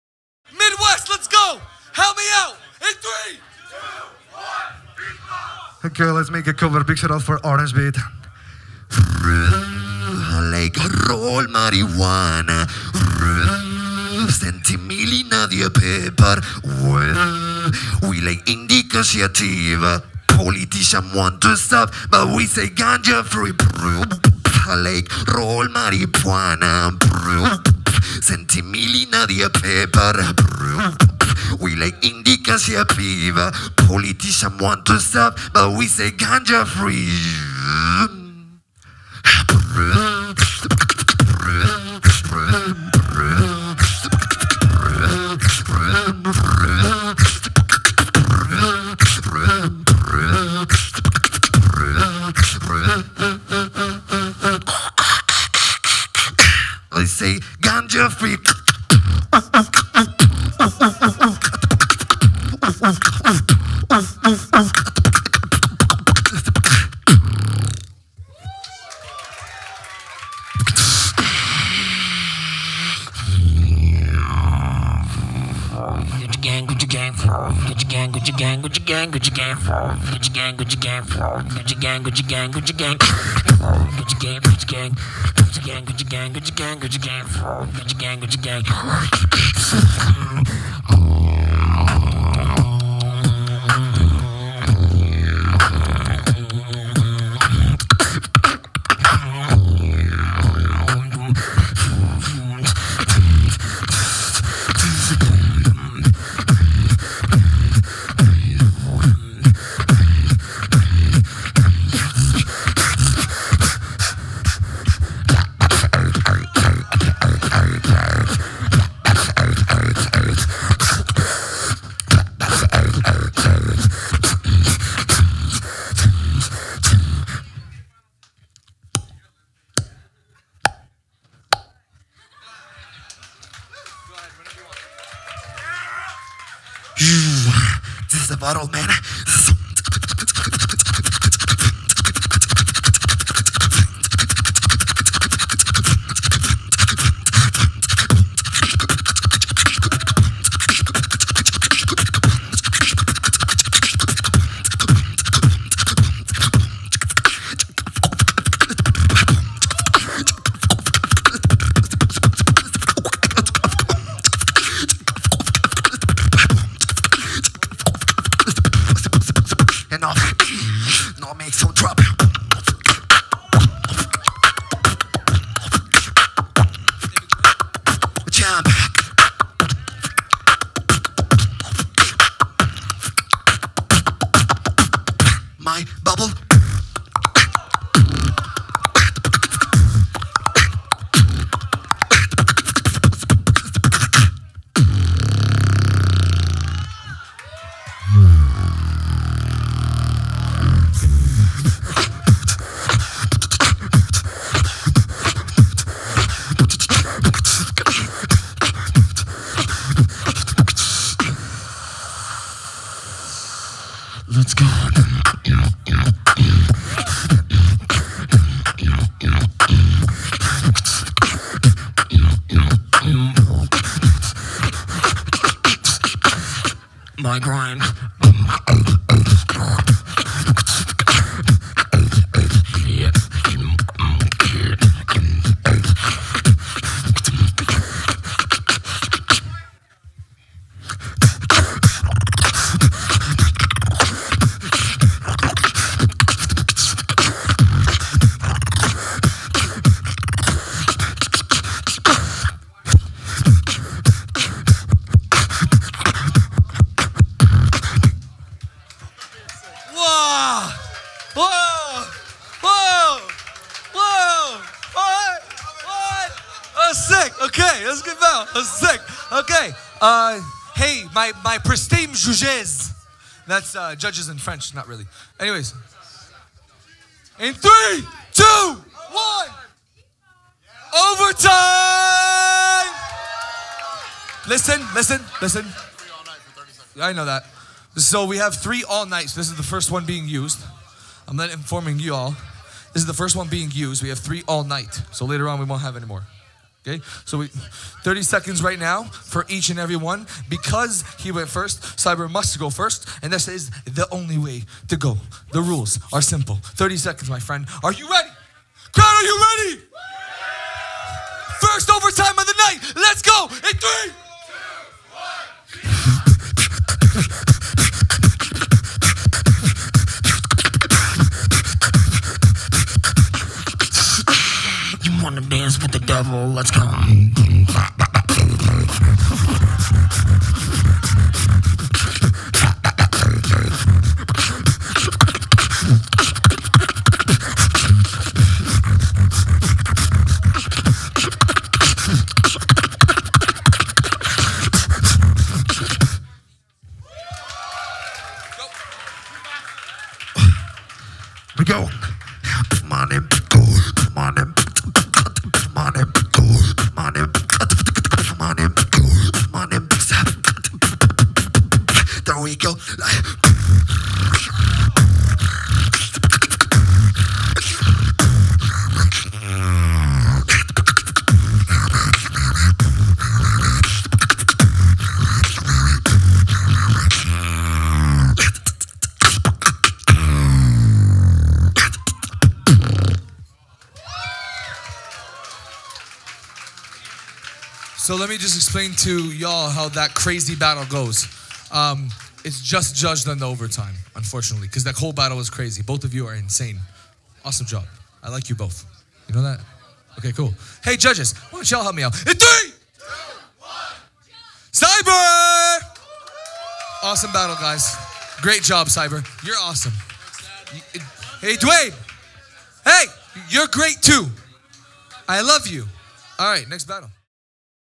Cyber vs Duade / Top 16 - Midwest Beatbox Battle 2017
Cyber and Duade met on stage for the top 16 round of the Midwest Beatbox Battle 2017.
The Midwest Beatbox Battle 2017
Organized by Shut Your Mouth and Beatbox.
Video production by Beatbox Television.
Event organization by Lethal FX.
Video dIrection by Adam Andrew Corre.
Camera Operation by Adam Andrew Corre, Kazu Iwai and Brian Schwartz.
Video editing by Adam Andrew Corre.
Sound mastering by Austin O' Brien.

Hosted by Lethal FX and BBK.
Jury featuring Mark Martin, Villain and Tony C.

Special thanks to Midwest Photo Exchange, Griffen Hollow Studio, Traxler Printing, Wild Goose Creative, HUMANBEATBOX.com.